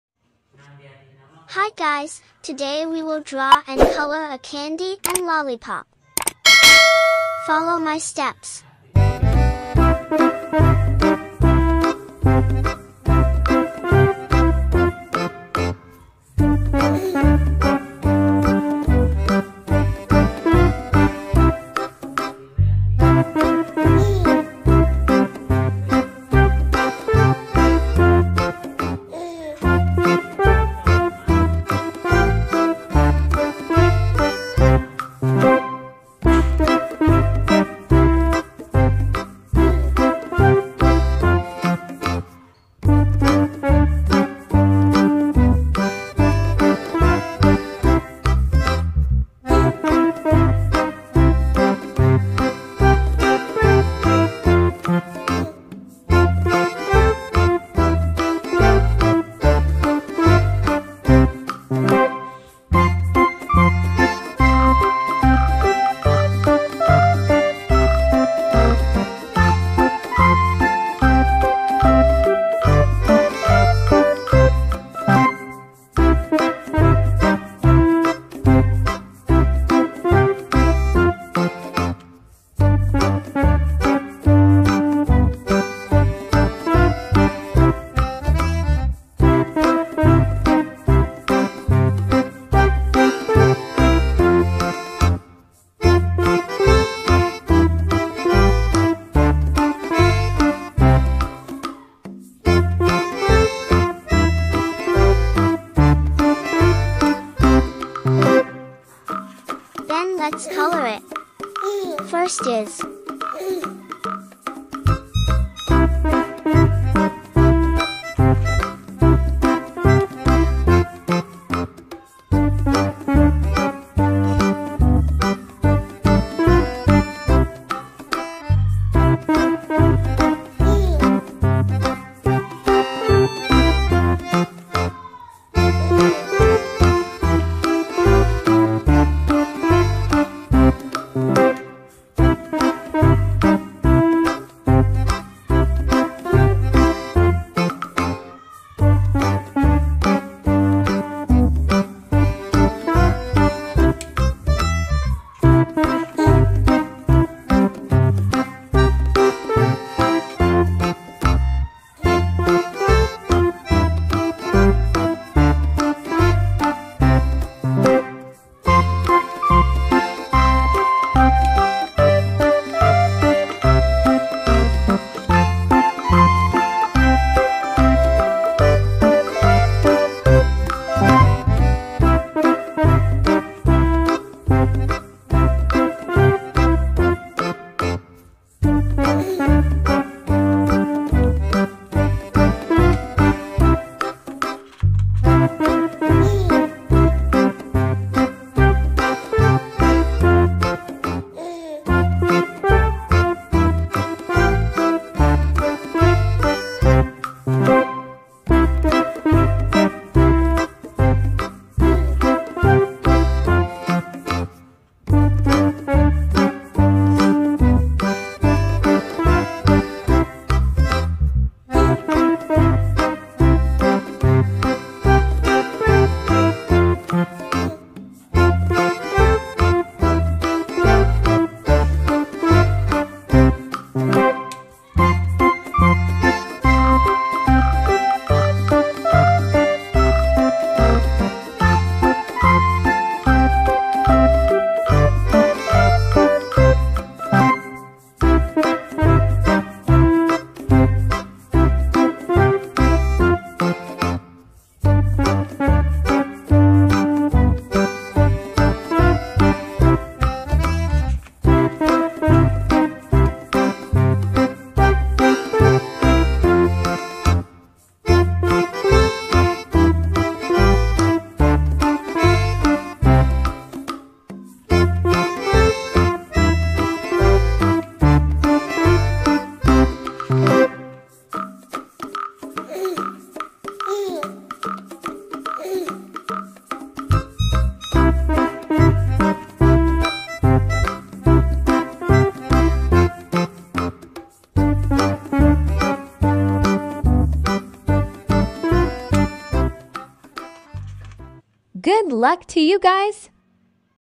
candy dan lolipop | drawing and coloring for children
In episode 33 we will learn how to draw and color candies and lollipops,  hope you like guys